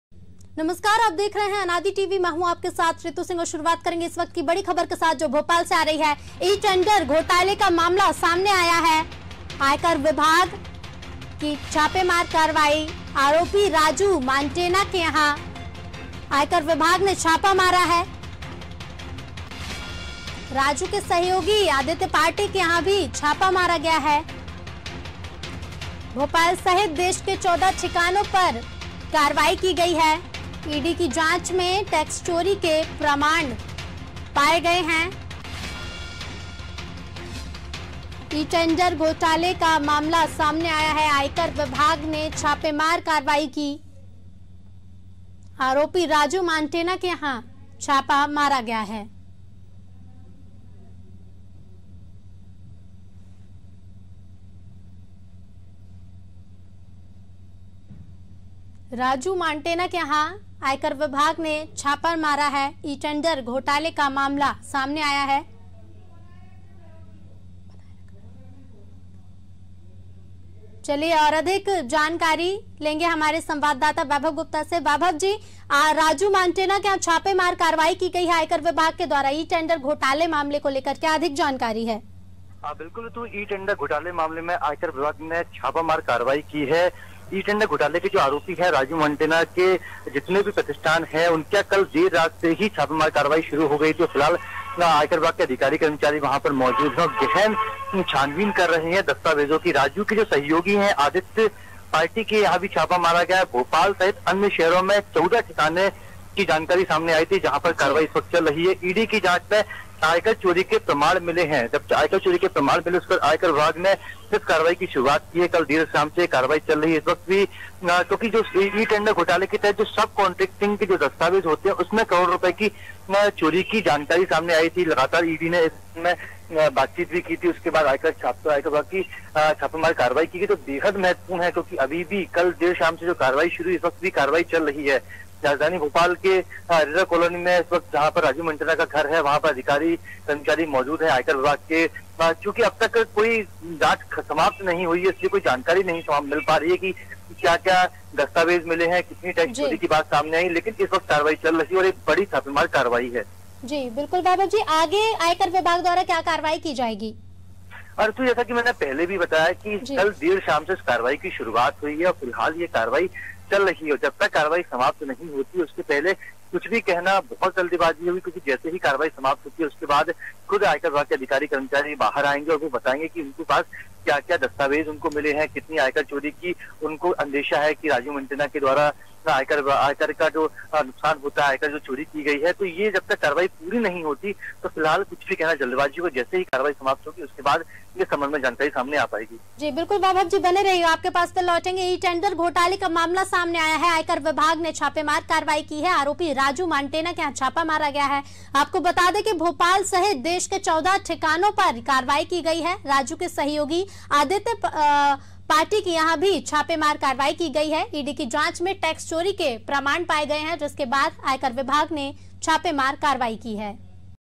भोपाल ई टेंडर घोटाला मामला |ANAADI BREAKING
भोपाल:- ई टेंडर घोटाला मामला आयकर विभाग का छापा आरोपी राजू मांटेना के यहां छापा भोपाल सहित 14 ठिकानों पर कार्रवाई